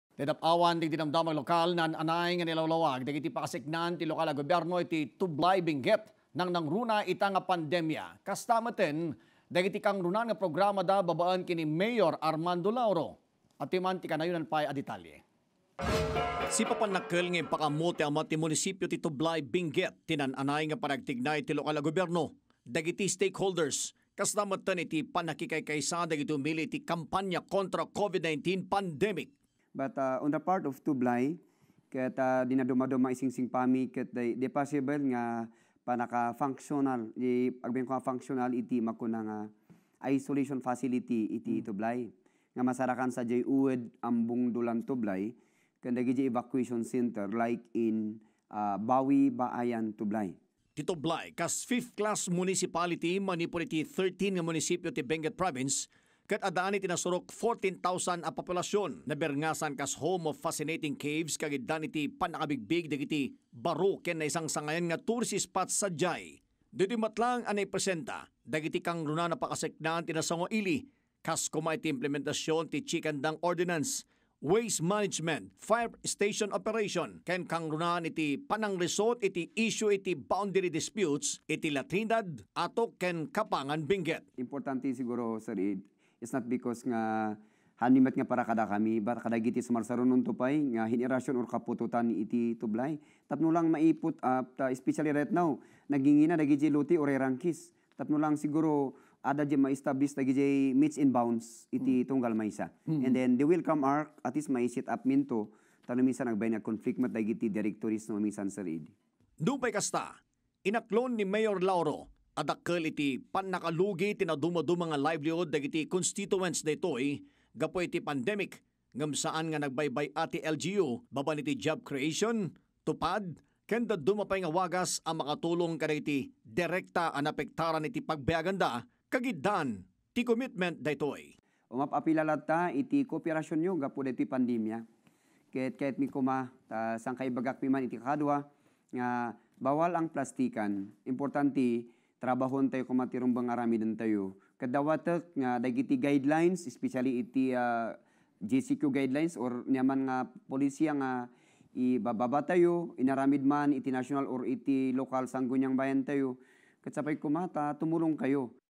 News: Kangrunaan a programa ken proyekto ti LGU Tublay, naipresenta
Naan-anay a nailawlawag dagiti pakaseknan ti lokal a gobyerno ti Tublay nangnangruna ita a pandemya kasta metten dagiti kangrunaan a programa da babaen kenni Mayor Armando Lauro.